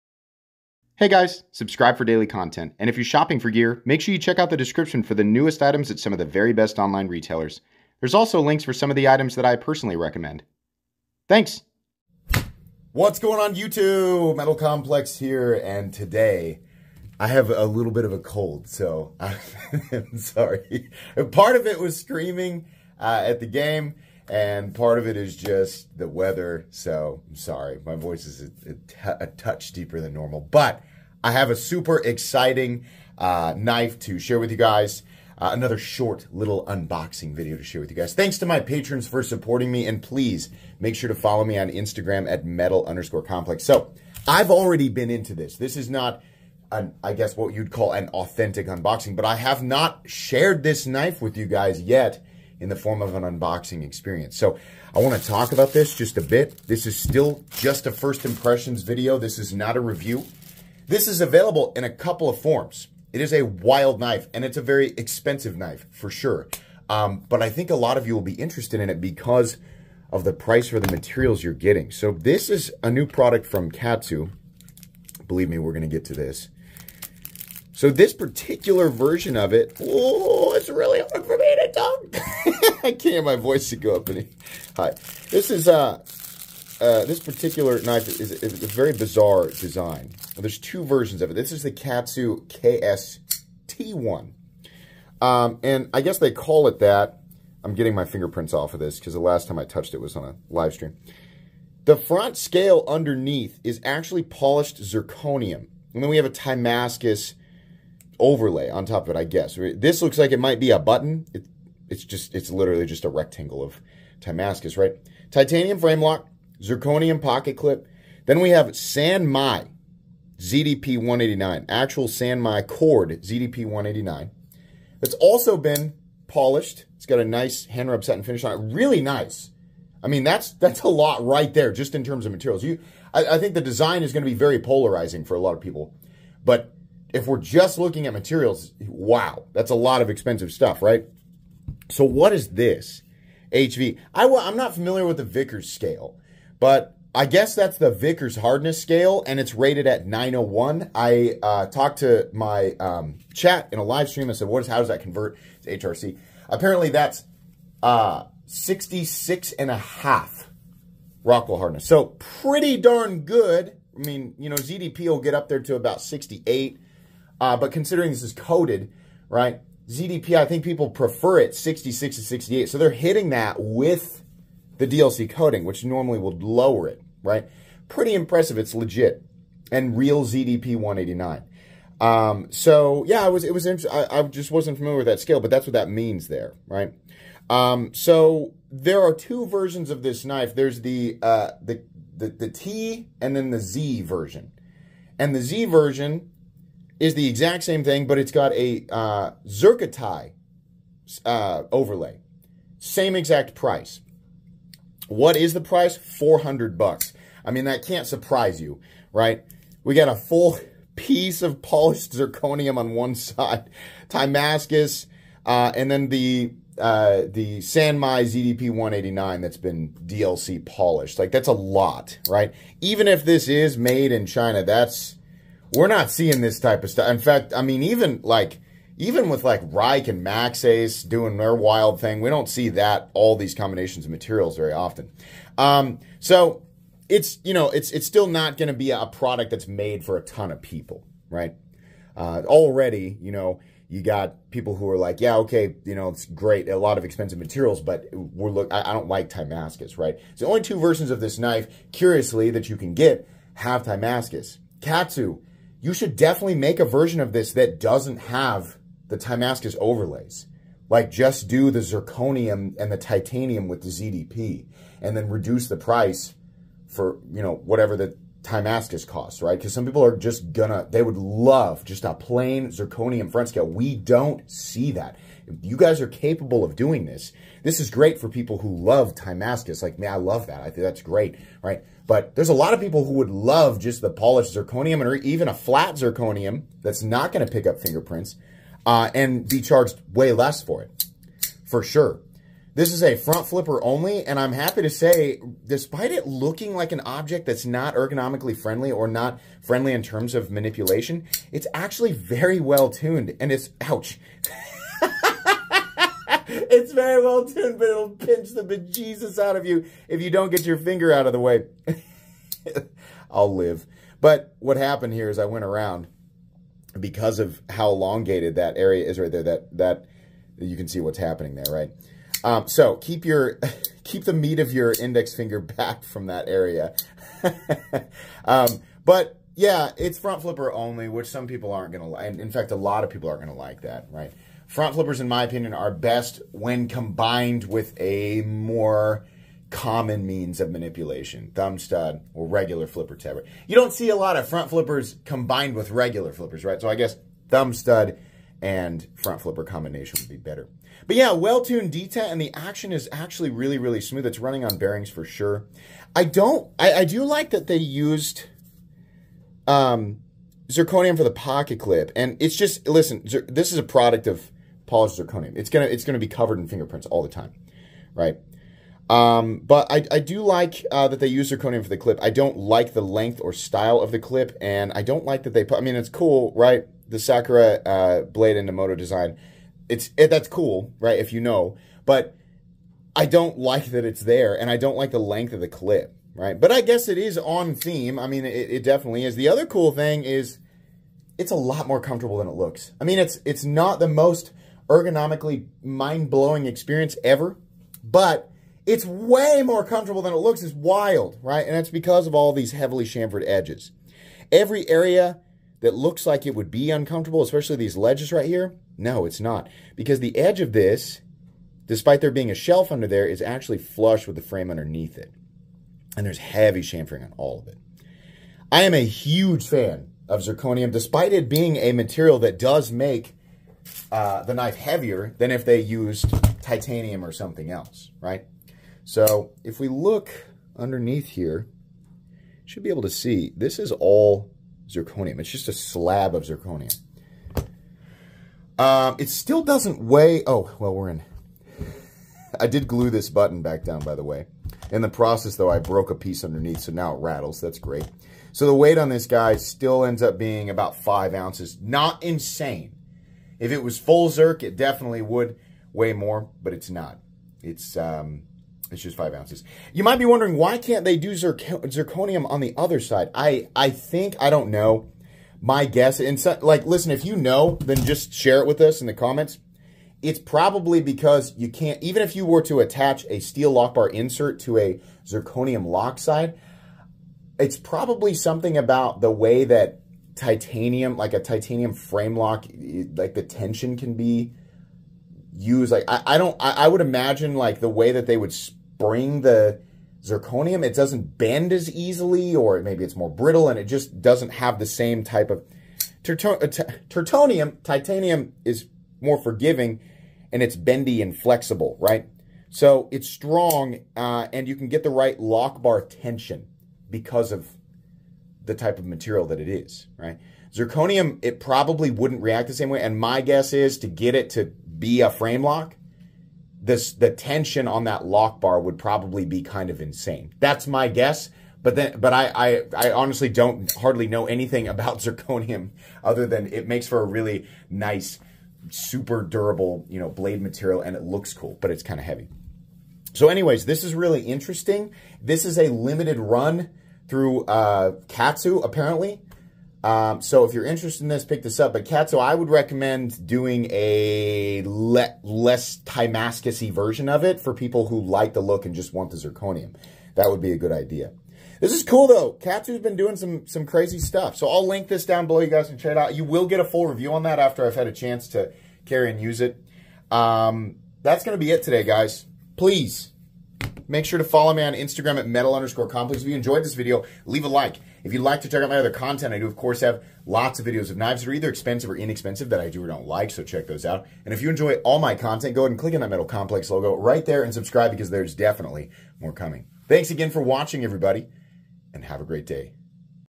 DO NOT STOP MAKING THESE KNIVES! ZIRCONIUM!?!?
Unboxing the Katsu KS-T1.

Polish Custom Knives New Arrivals!

Check out Tools For Gents!

Shop Flytanium! Custom parts for your favorite knives!